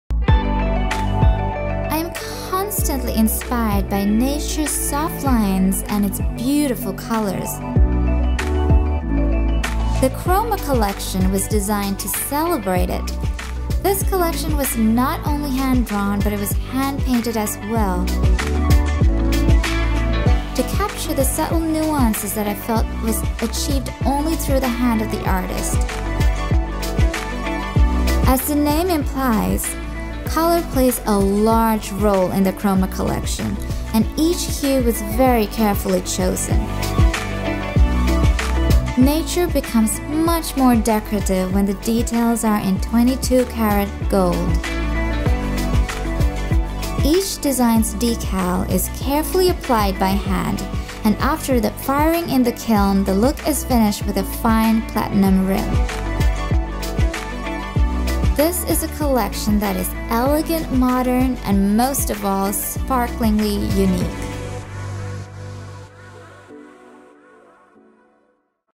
Taprobaine Dinnerware - Chroma Collection
Watch our stunning Chrome Collection dessert plates come to life.

Bring art into your home with hand-painted luxury dinnerware sets
The Chroma Flora fine china dinnerware set celebrates the beauty of nature with 22 karat gold finishes.